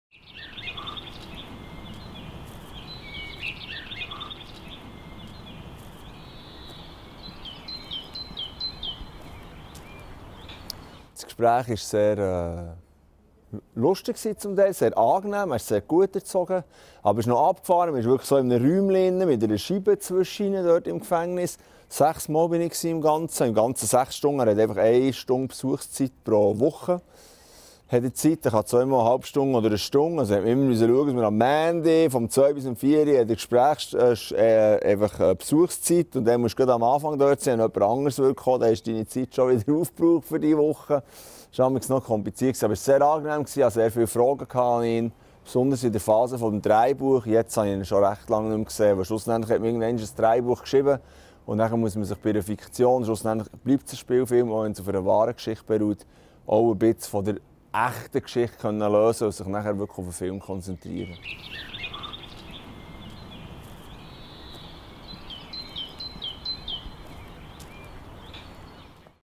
Der Fall Peter K. - Laurent Wyss im Interview Teil 6
Der Regisseur Laurent Wyss spricht über den Besuch bei Peter K. im Gefängnis.

Erstellt von Patricia Suter.